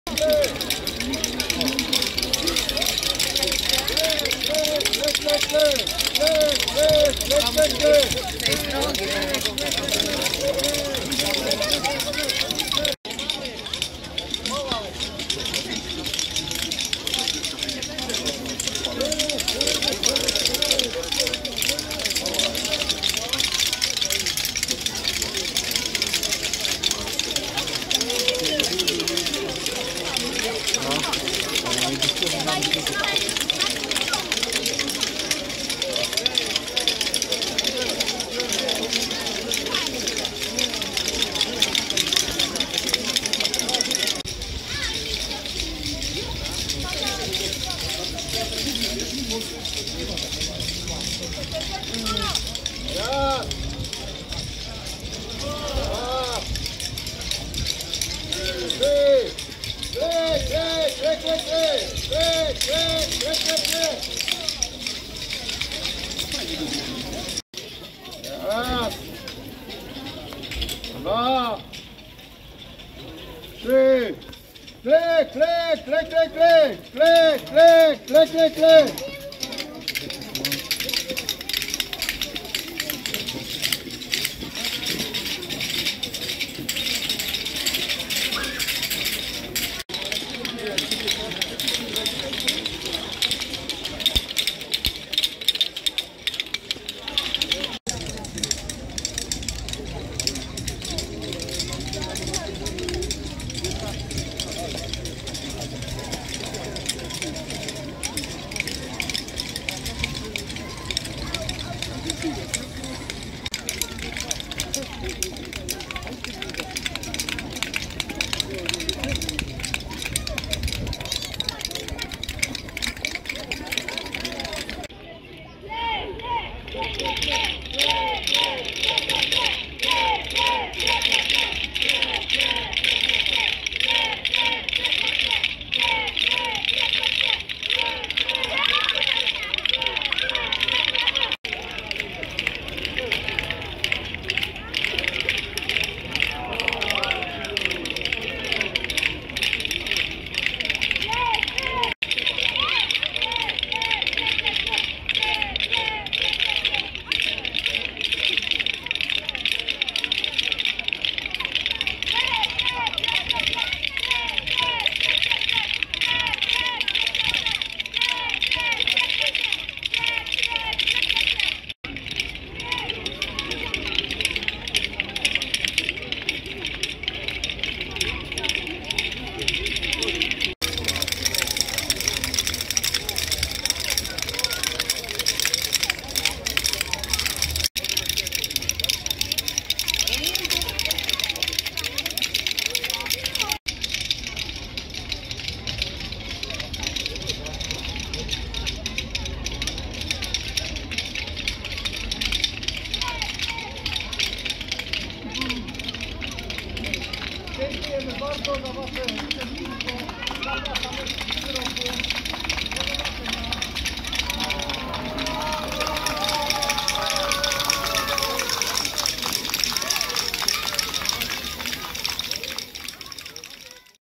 Pochód z Judoszem
W Wielką Sobotę 8 kwietnia Judosz znowu przeszedł ulicami Skoczowa, a następnie został spalony za strażnicą OSP.